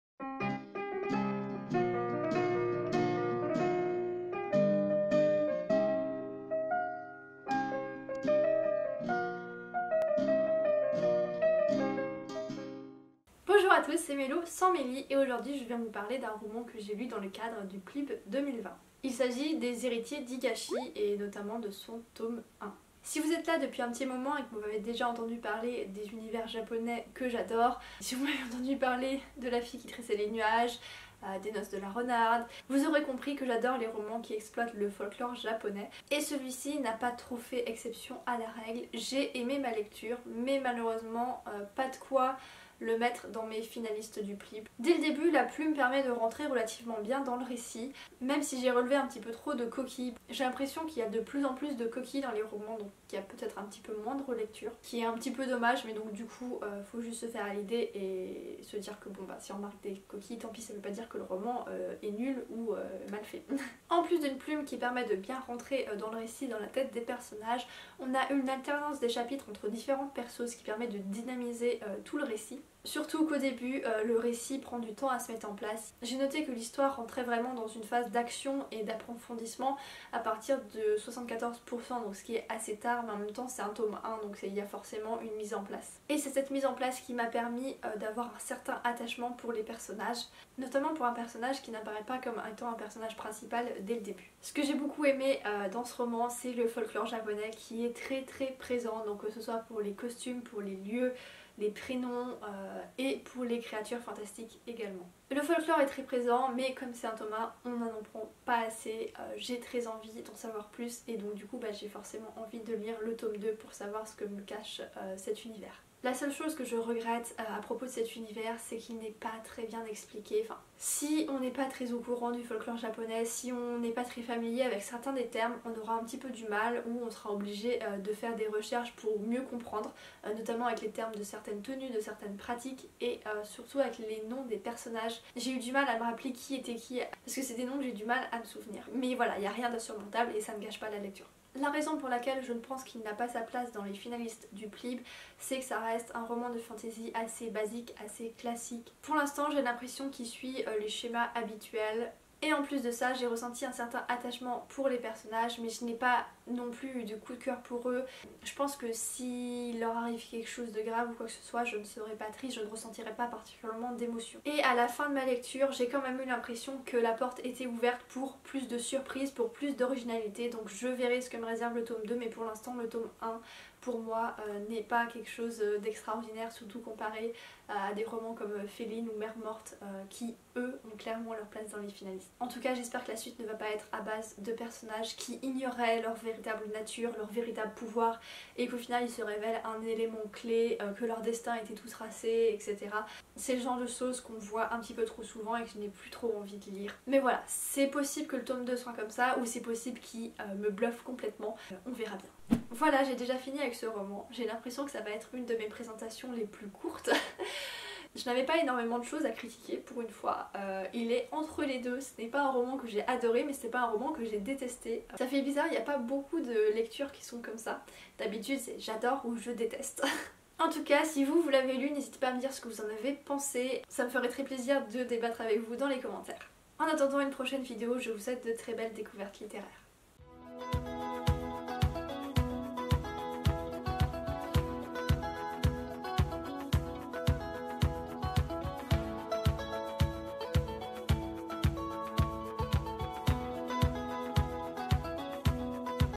📗 Présentation PLIB2020 | Les héritiers d'Higashi
Hello, c'est Mélo !

Ces derniers temps, j'ai fait beaucoup de lecture dans le cadre du PLIB2020, Les Héritiers d'Higashi en fait partie ! Ce fut une bonne lecture sans plus, qui me donne néanmoins envie de découvrir ce que la suite nous réserve !

Vous l'avez lu ? Il est dans vos finalistes ?

Musique proposée par La Musique Libre